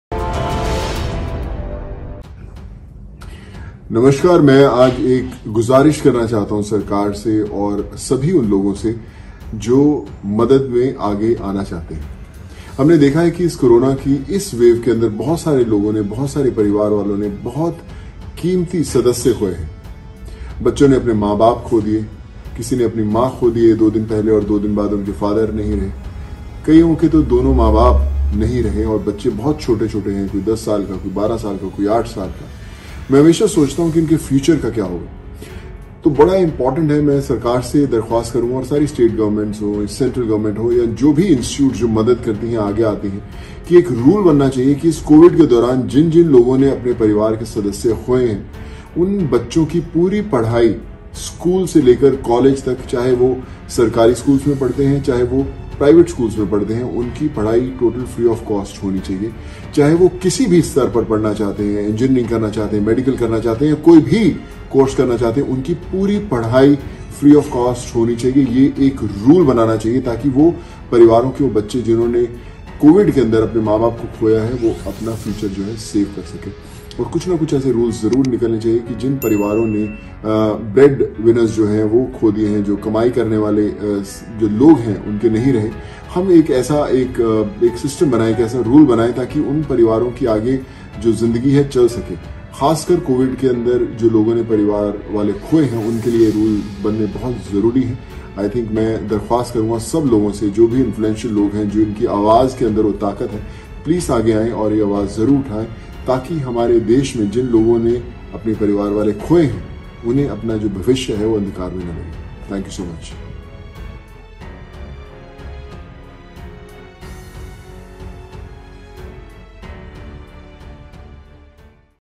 sonu Sood & COVID-19 |sonu Sood ne kya apeal ki Sarkar se |kya sarkar puri karegi sonu Sood ki apeal
kaise madad ki soon Sood ne logo ki Covid-19 me
kya apeal kiya sonu Sood ne sarkar se
kya sarkar manegi sonu Sood ki baat